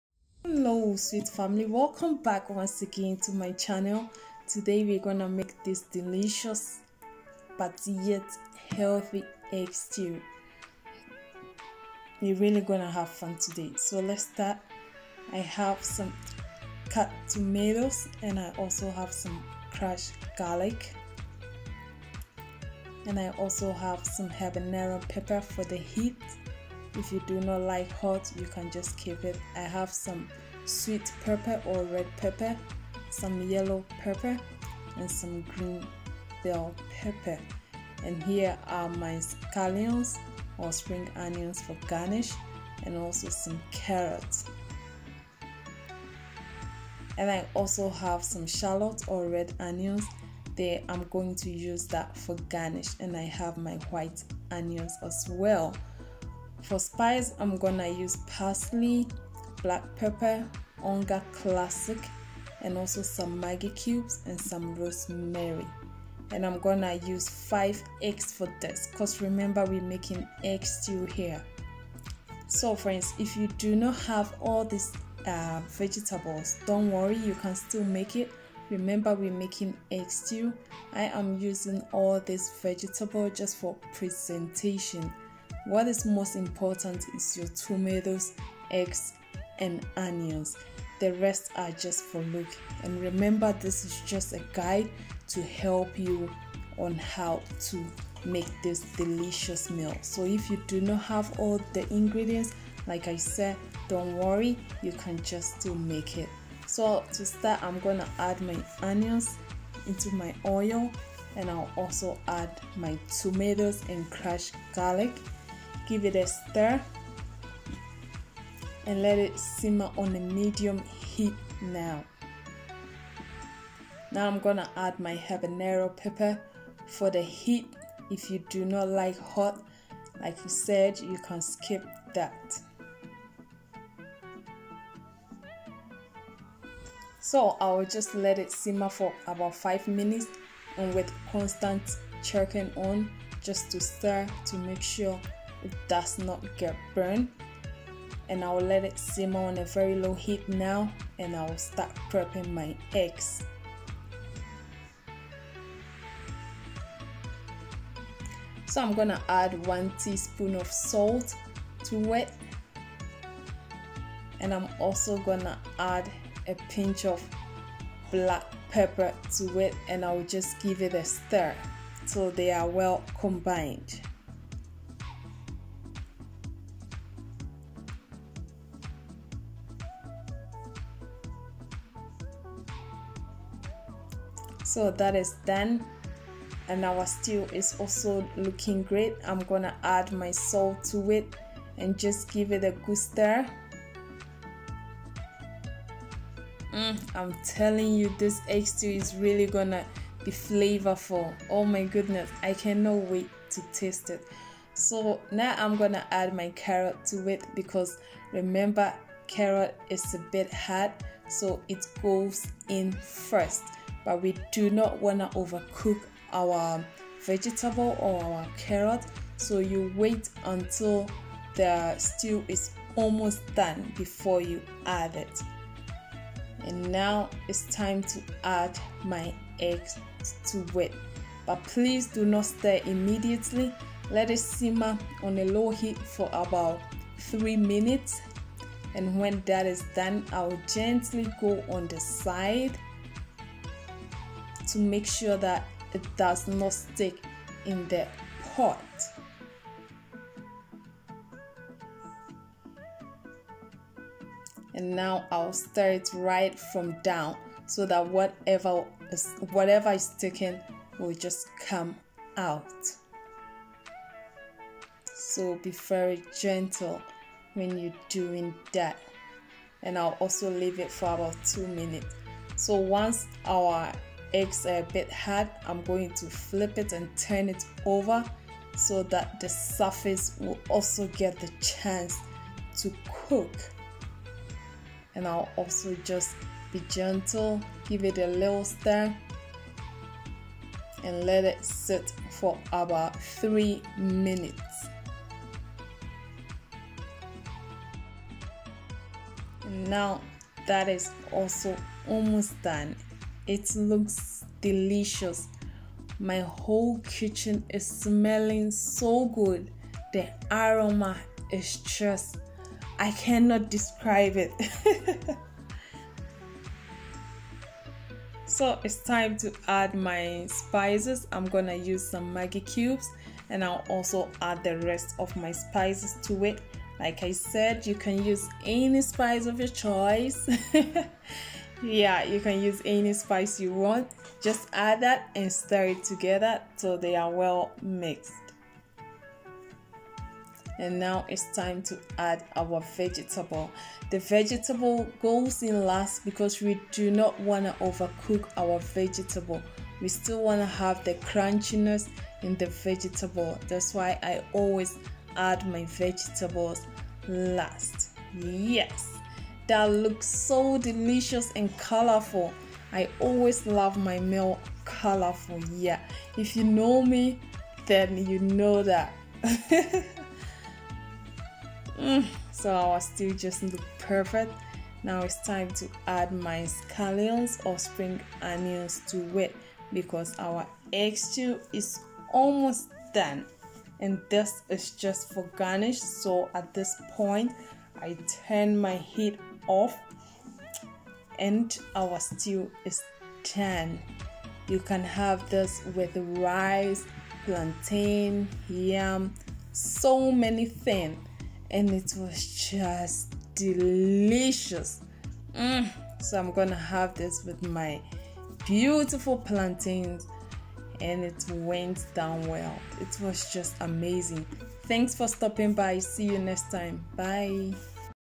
HOW TO MAKE EGGS STEW.
Hello there sweet fam welcom back again let's cook some delicious Eggs Stew

INGREDIENTS

5 Eggs
Oil
Tomatoes
Onions
Habenaro pepper
Salt
Rosemary
Spice of your choice

OPTIONAL INGREDIENTS

Spring onions
Carrots
Green pepper
Red/Sweet pepper
Green pepper.